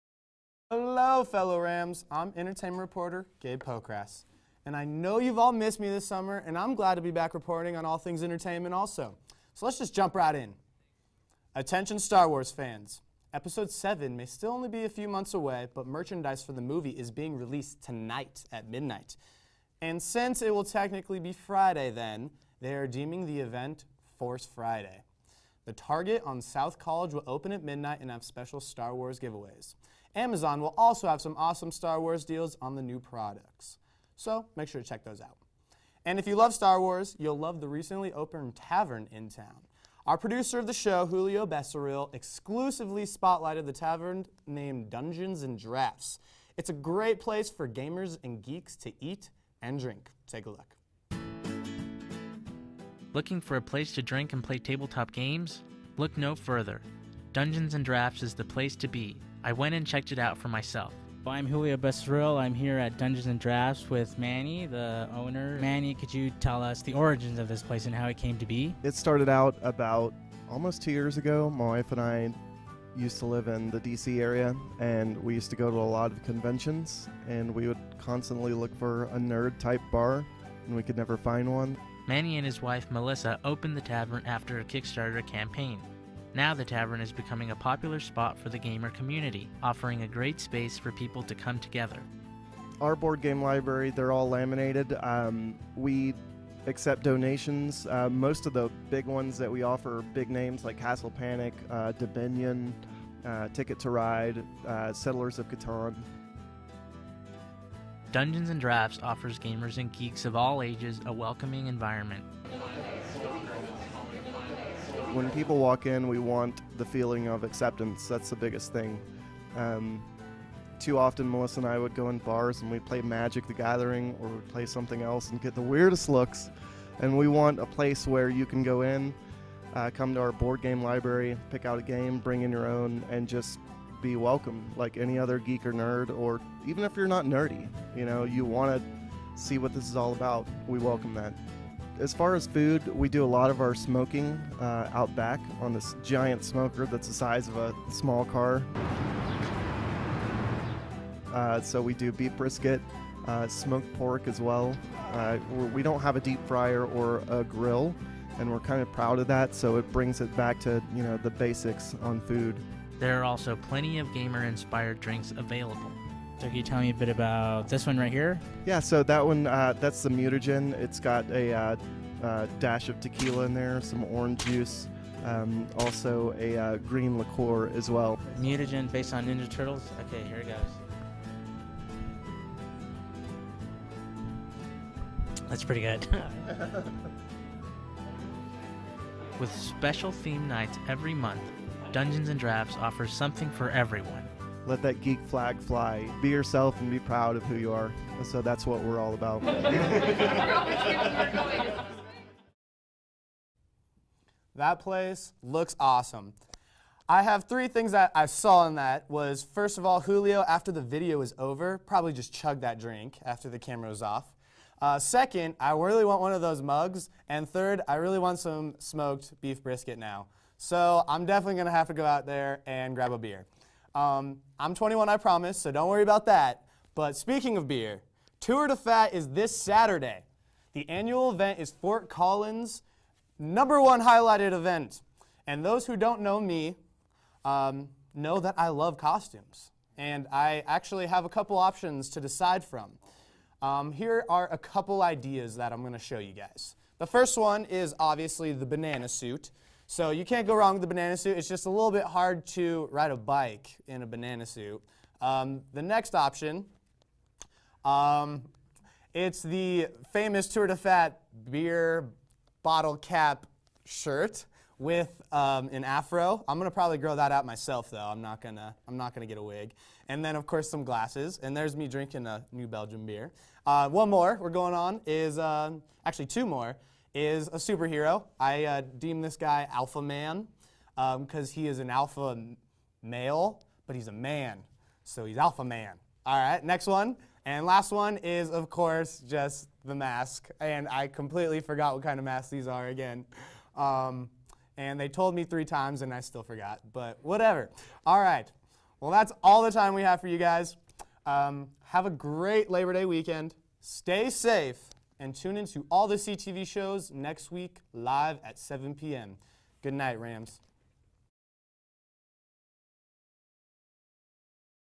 CTV Entertainment Segment: Thursday, Sept. 3, 2015
Gabe Pocrass shares some of his costumes ideas for Tour de Fat and producer Julio Becerril visits Dungeons and Drafts.

Watch CTV 11 live on Comcast Channel 11 or stream it on the collegian.com app.